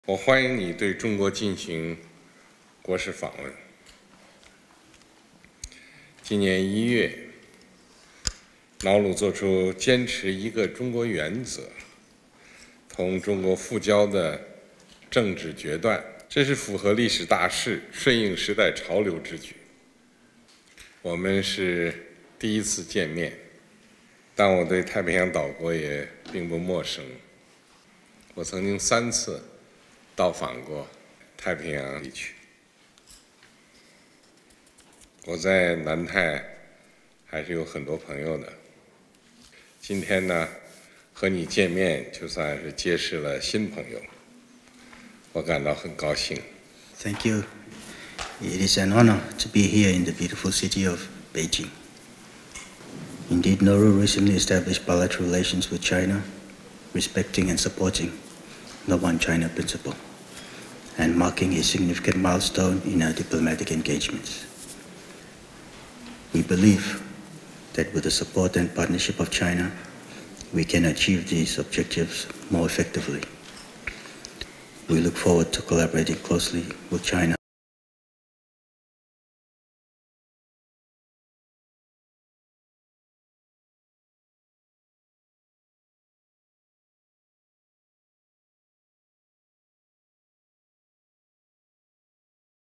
Xi Calls Nauruan President as New Friend from South Pacific
Chinese President Xi Jinping called visiting Nauruan President David Adeang a good friend from the South Pacific region when holding talks with him in Beijing on Monday.